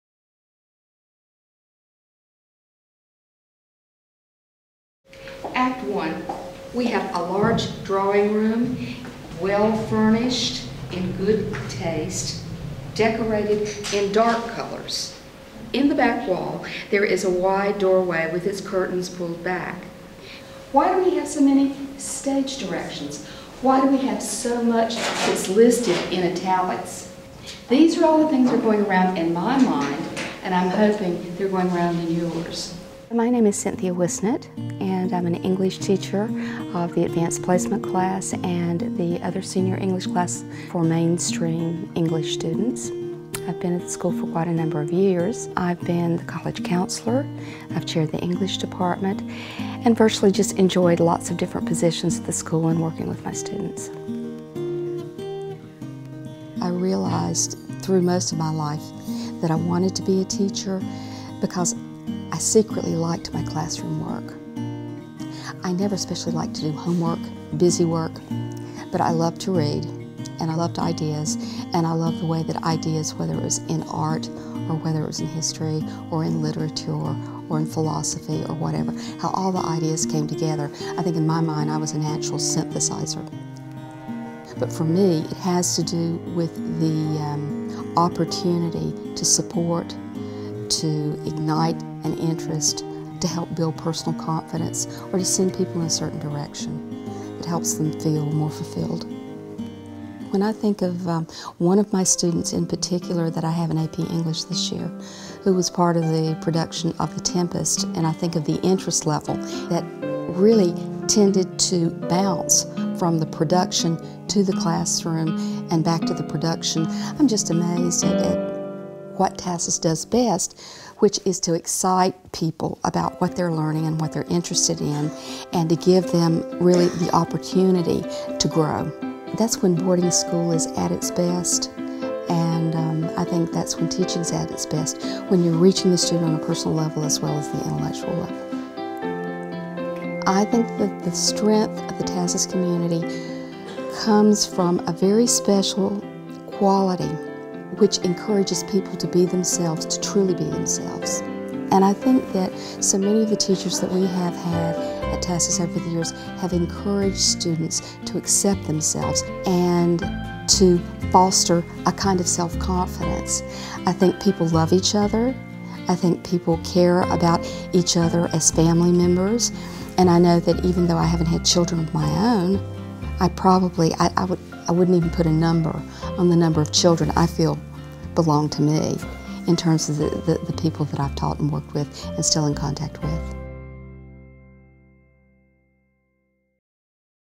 Faculty Profiles: Cynthia Whisenant
TASIS veteran Cynthia Whisenant reflects on what it means to be a teacher at TASIS.